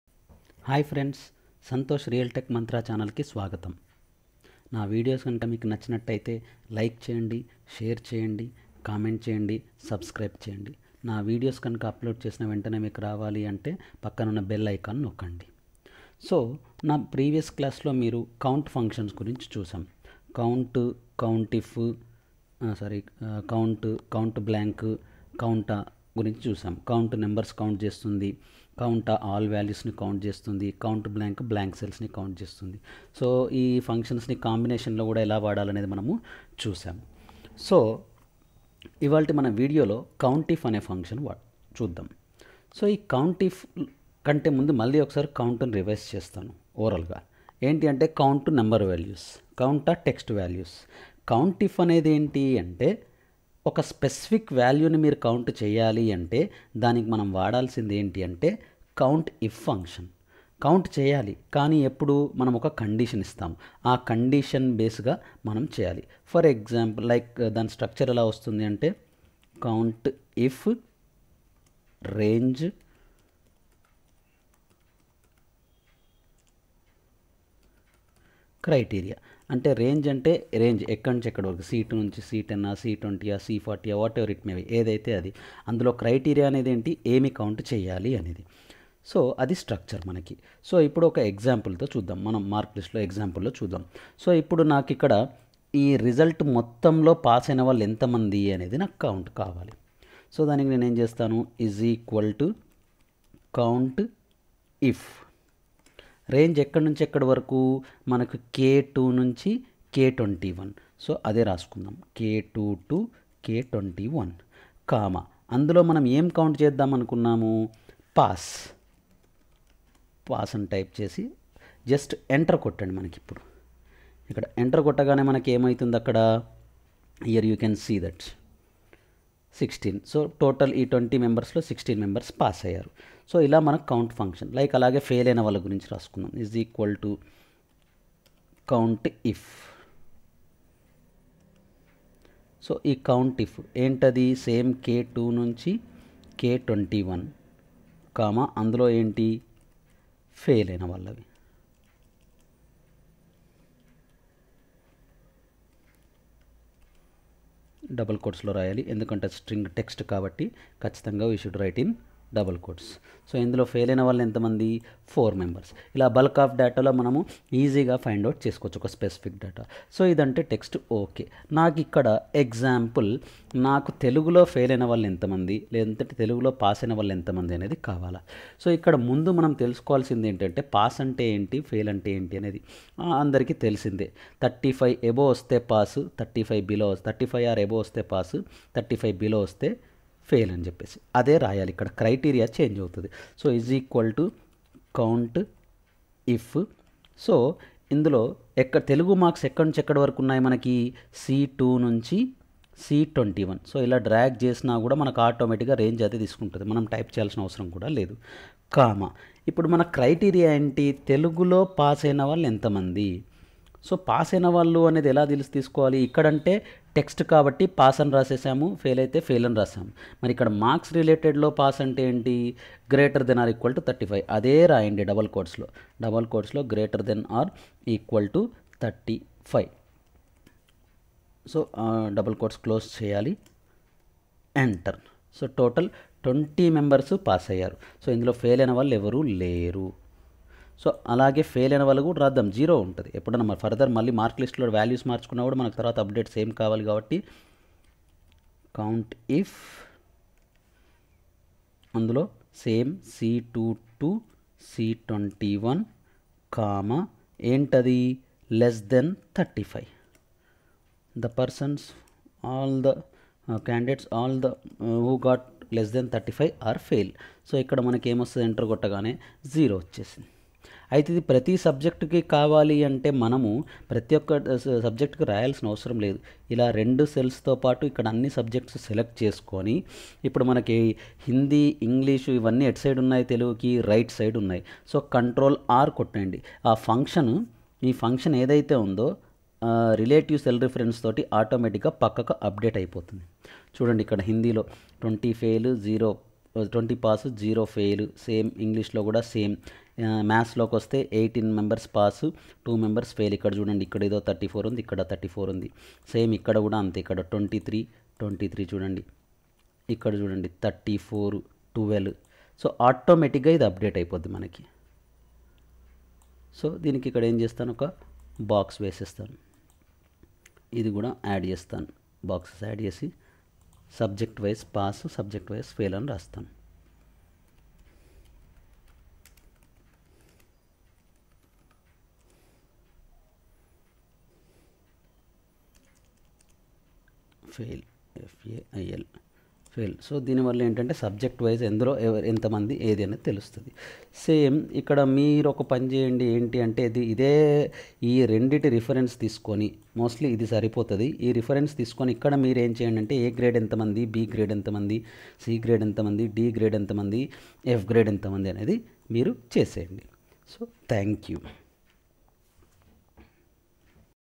How to Count Specific Values in Excel
In this educational video, we will guide you on how to count specific values in Excel using the Countif function. Whether you're a beginner or an experienced user, this tutorial will provide you with the tools to automate your data analysis by accurately counting specific values with ease. Expand your Excel skills and improve your productivity by watching now. Don't miss out on this opportunity to level up your spreadsheet game